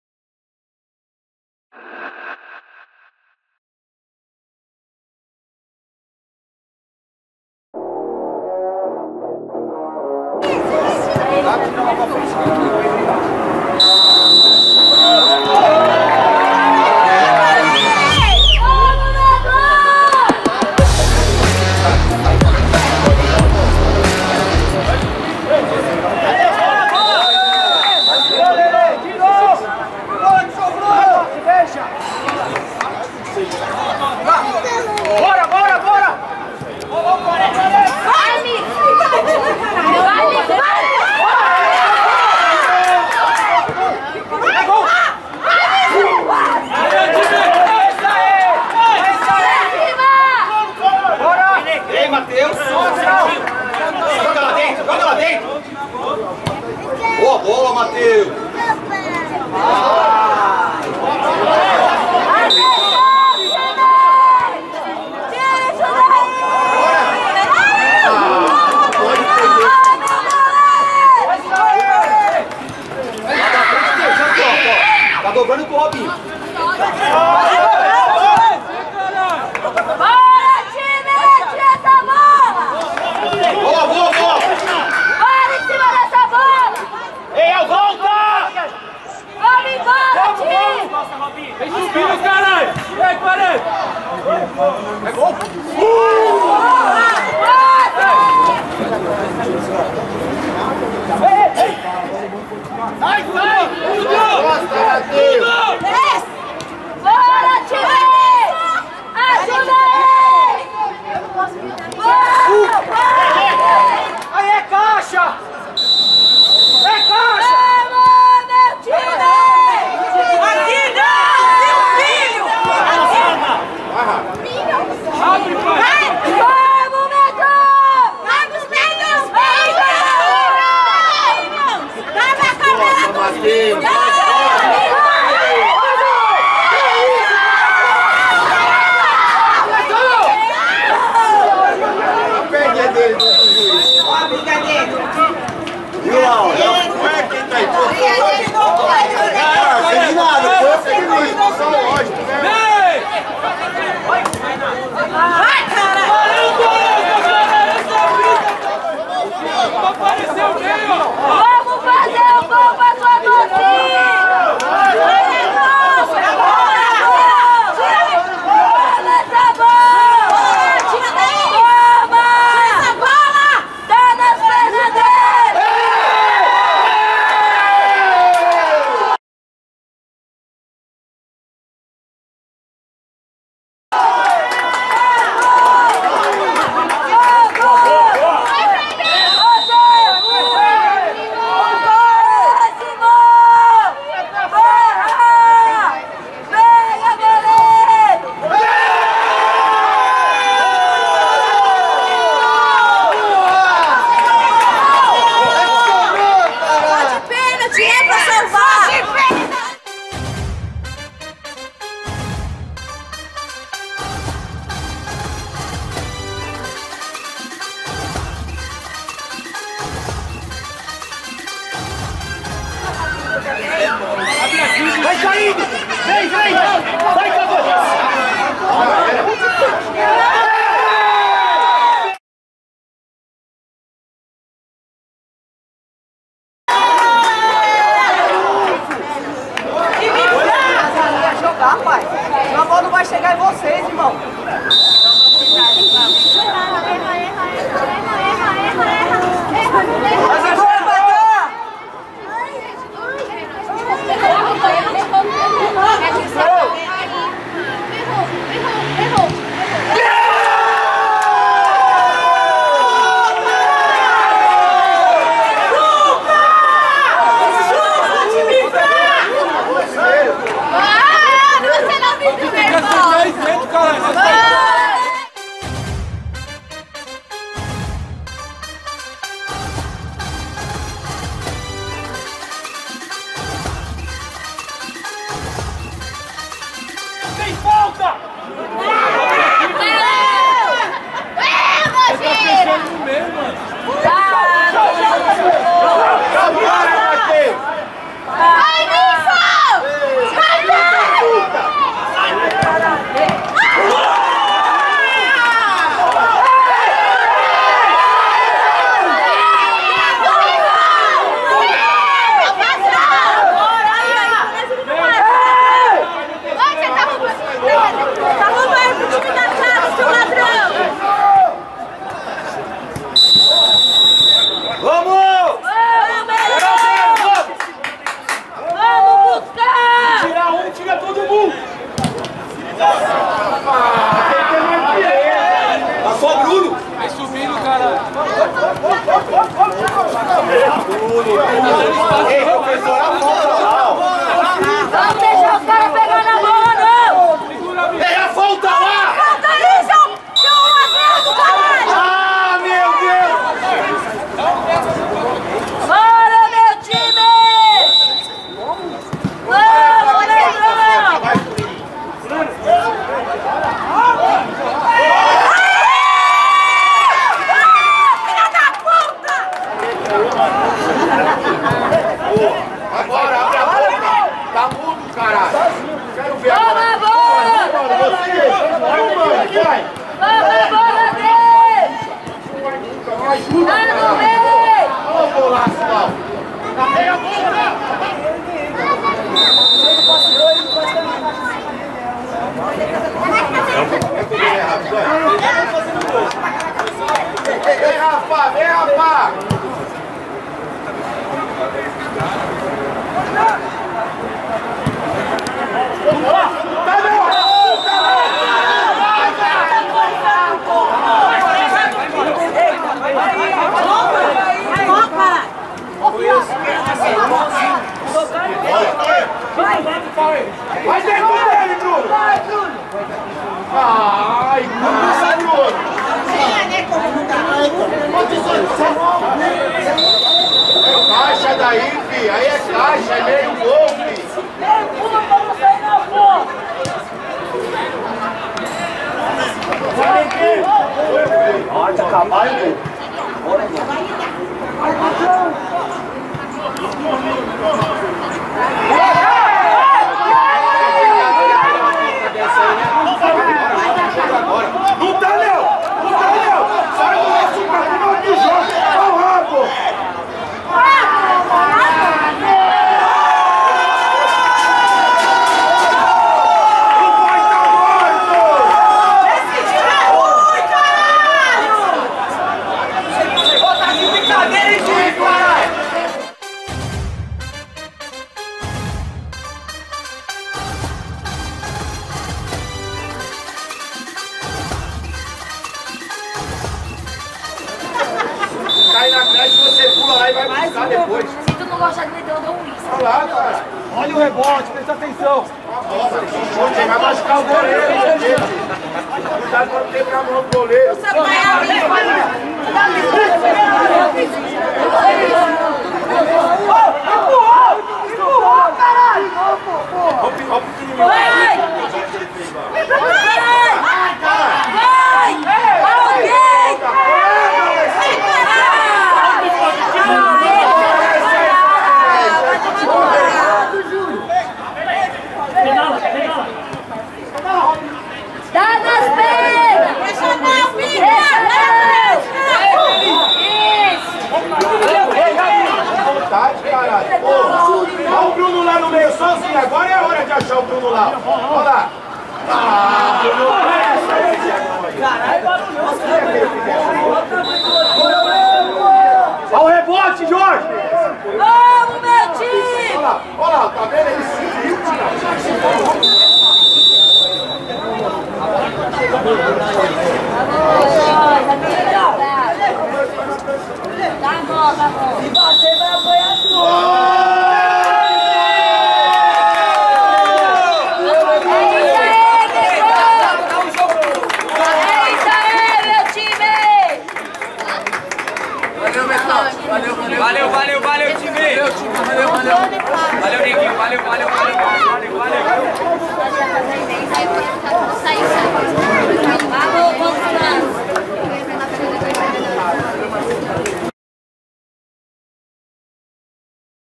#3 SUPER FESTIVAL 2024 - KI SOBROU FC x METRÓPOLES B.V
SUPER FESTIVAL 2024 - SUPER LIGA PAULISTA
KI SOBROU FC x METRÓPOLES B.V
CATERGORIA: 18+